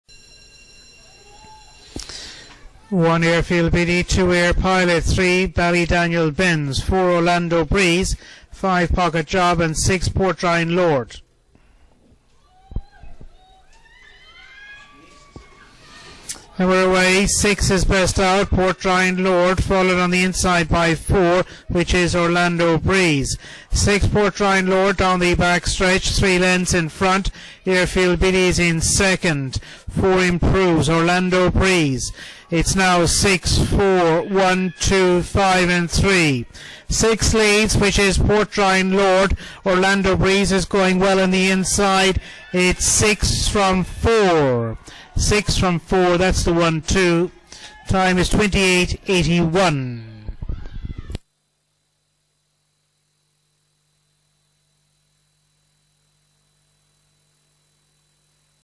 IGB - Good News @ Stud A1 24/11/2018 Race 11 - Limerick
Race: Good News @ Stud A1
Track Name: Limerick
Date: 24/11/2018
Hour: 22:09

Results

1st    6     PORTDRINE LORD      7/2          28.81
2nd    4     ORLANDO BREEZE      4/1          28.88
3rd    5     POCKET JOB          4/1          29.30
4th    2     AIR PILOT           3/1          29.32
5th    3     BALLYDANIEL BENZ    2/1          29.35
6th    1     AIRFIELD BIDDY      7/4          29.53

|LMK_2018_11_24_11